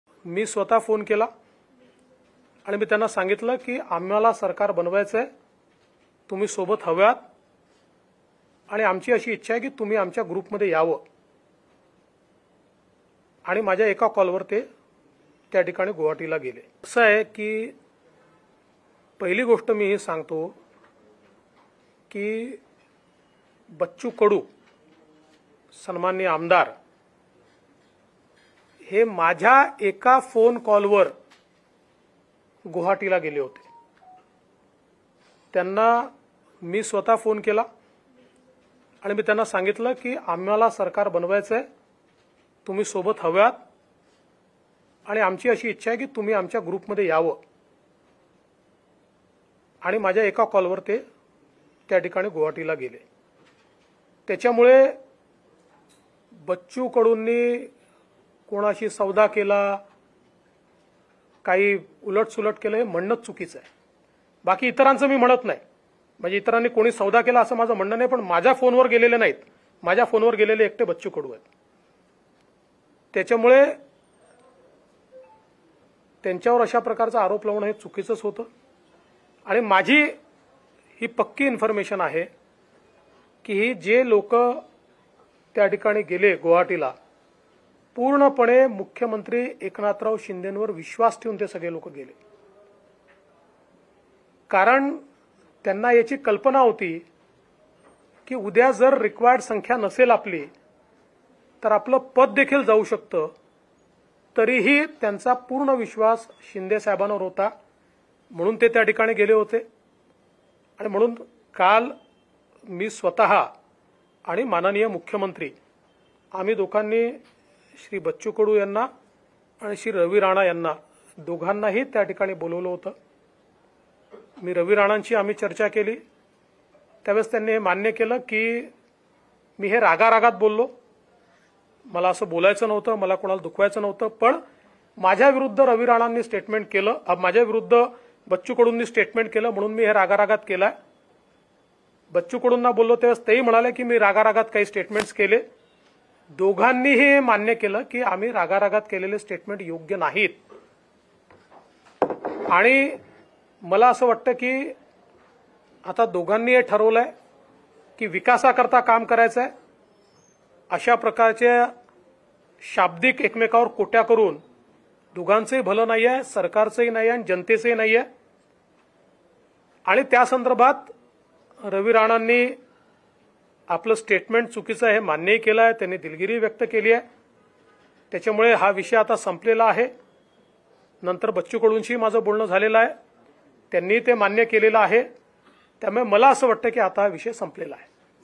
Devendra Fadnavis on Ravi Rana | देवेंद्र फडणवीसांनी रवी राणांना सुनावलं | Maharashtra Times
बच्चू कडू आणि रवी राणा यांच्यातील वादावर देवेंद्र फडणवीस यांनी भाष्य केलं.माझ्या एका कॉलवर बच्चू कडू गुवाहाटीला गेले असं फडणवीस म्हणाले. बच्चू कडूंनी कुणाशी सौदा केला असं म्हणणं चुकीचं आहे असं फडणवीस म्हणाले. बच्चू कडू आणि रवी राणा यांच्यात मध्यस्थी करून वाद मिटल्याचं फडणवीसांनी सांगितलं.